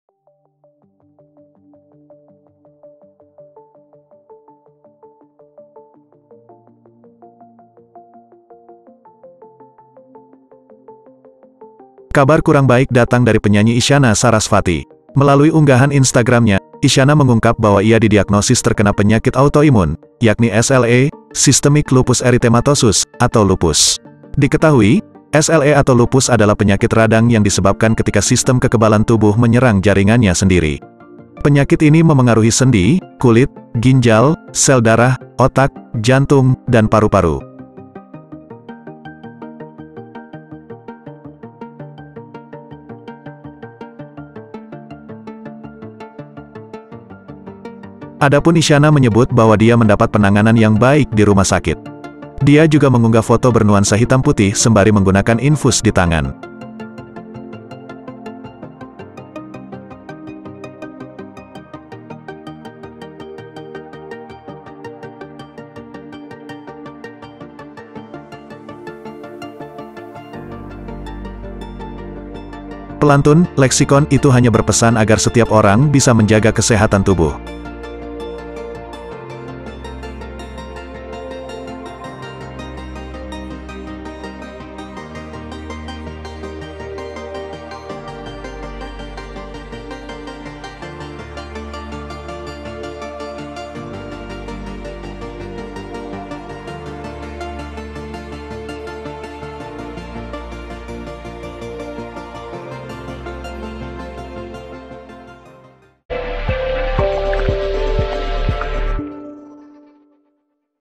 Isyana Sarasvati Ungkap Didiagnosis Idap Autoimun Lupus
Kabar kurang baik datang dari penyanyi Isyana Sarasvati. Melalui unggahan Instagram-nya, Isyana mengungkap bahwa ia didiagnosis terkena penyakit autoimun, yakni SLE (Systemic Lupus Erytematosus) atau lupus.

Diketahui, SLE atau lupus adalah penyakit radang yang disebabkan ketika sistem kekebalan tubuh menyerang jaringannya sendiri. Penyakit ini memengaruhi sendi, kulit, ginjal, sel darah, otak, jantung, dan paru-paru. (ami)